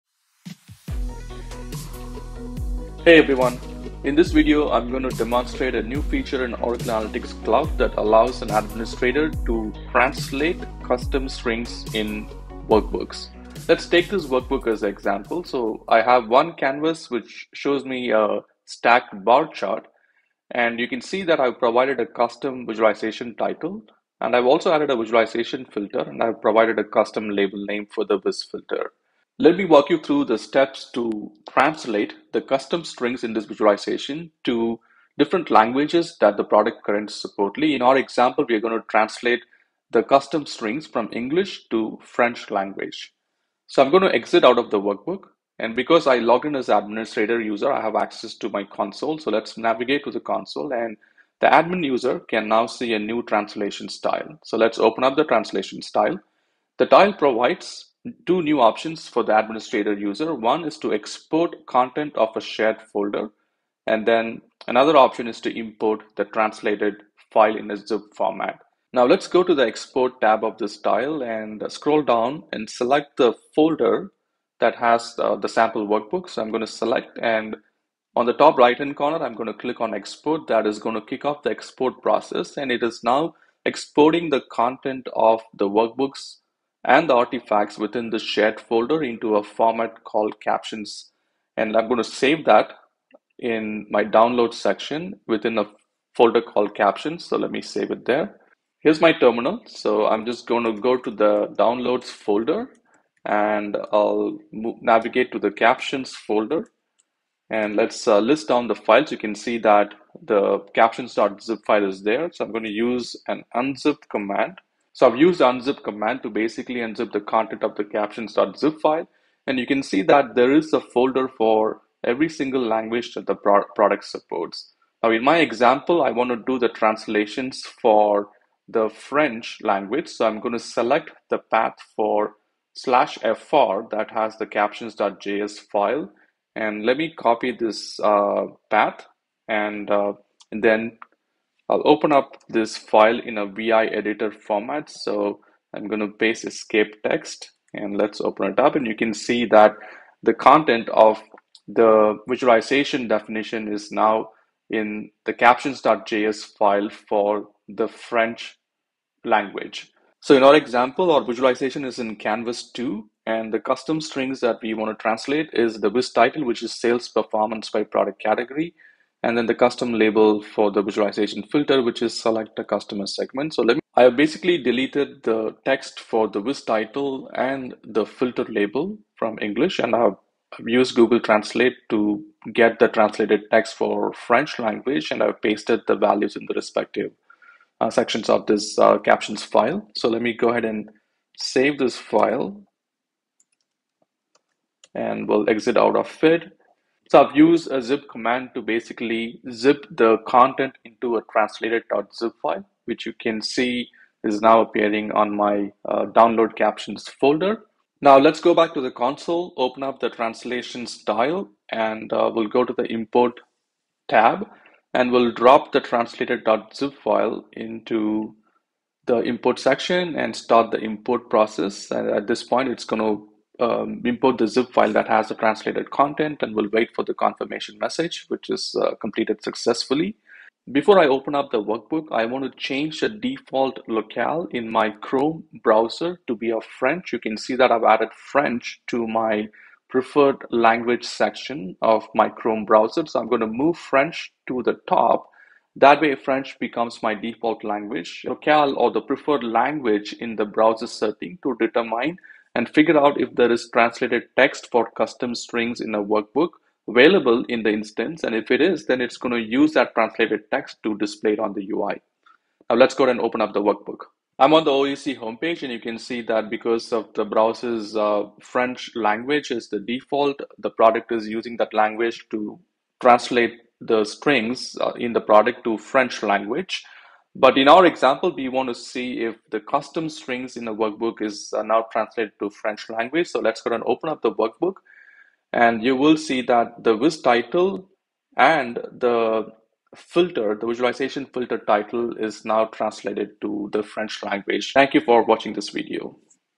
Translation of Workbooks Artifacts in Oracle Analytics
This video shows how to configure translation for workbooks artifacts (Viz Titles, Axis Labels, etc ...)  in Oracle Analytics Data visualizations . This capability is part of the Oracle Analytics Cloud January 2024 update.

A playlist of more of the new features coming with January 2024 OAC update is available